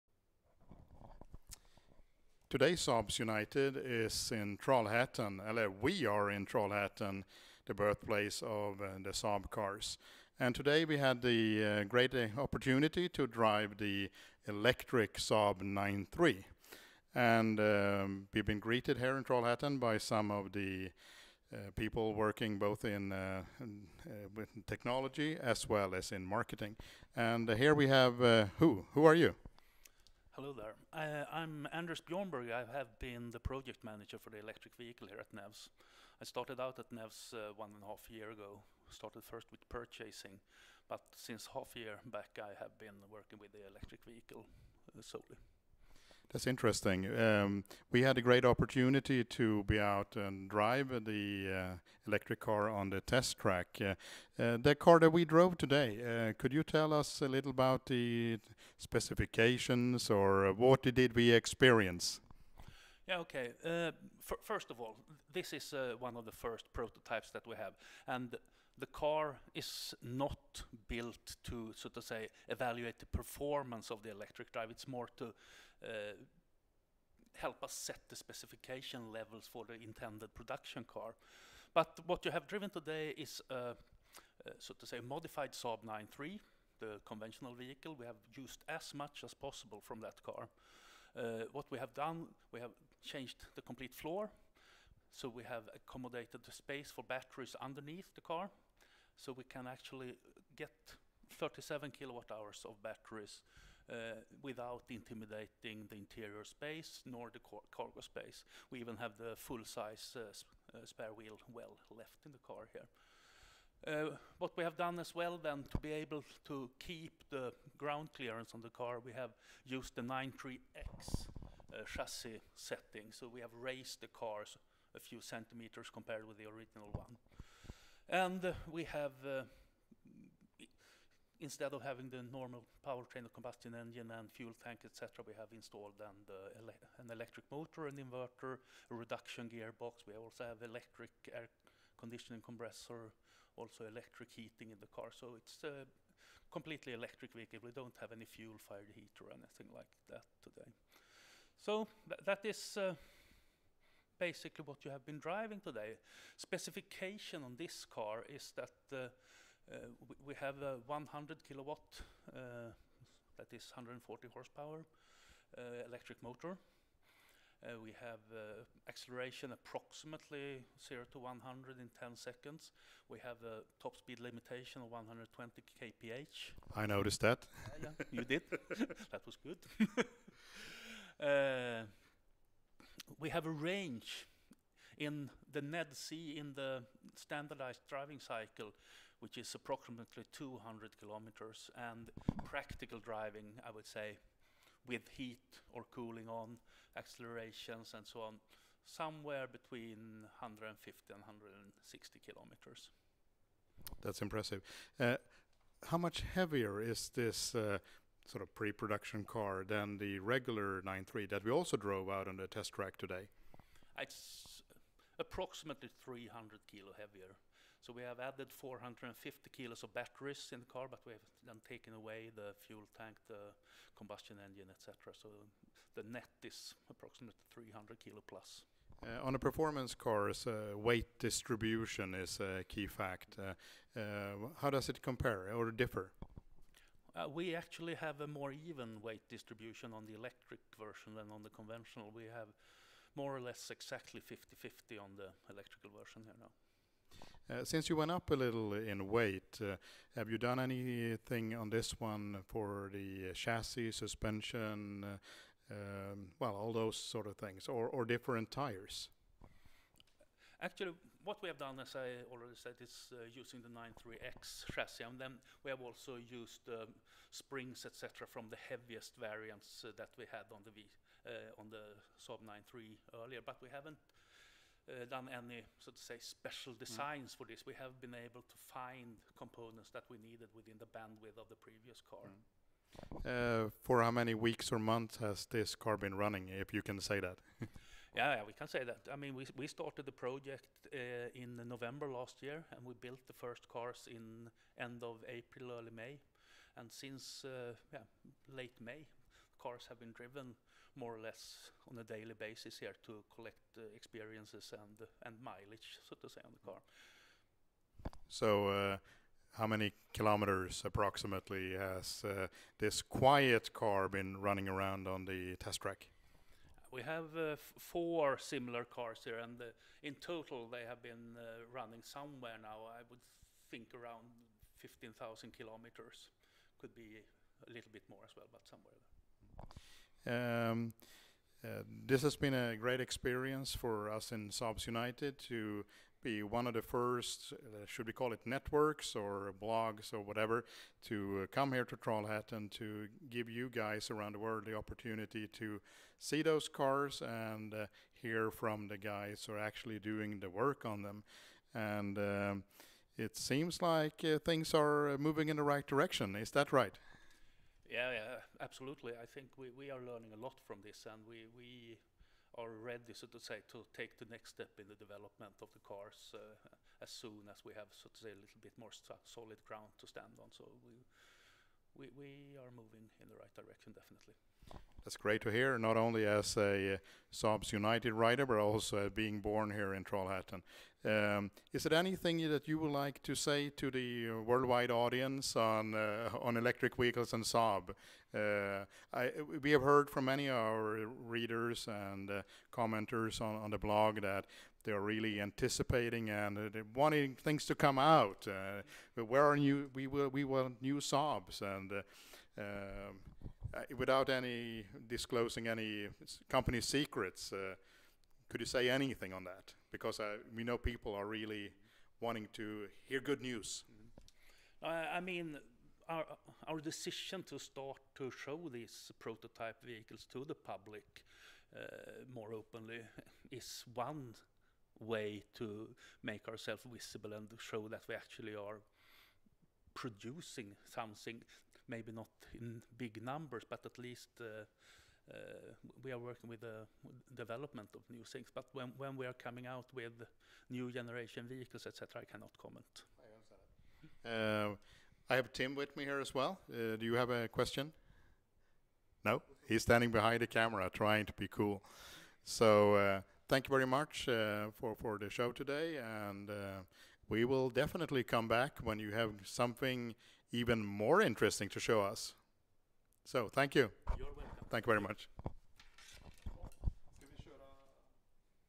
Saab 9-3 Electric Car Interview with NEVS in Trollhättan
Brief interview with representative of NEVS after a testdrive of the all electric Saab 9-3 at the secrecy Saab Test Track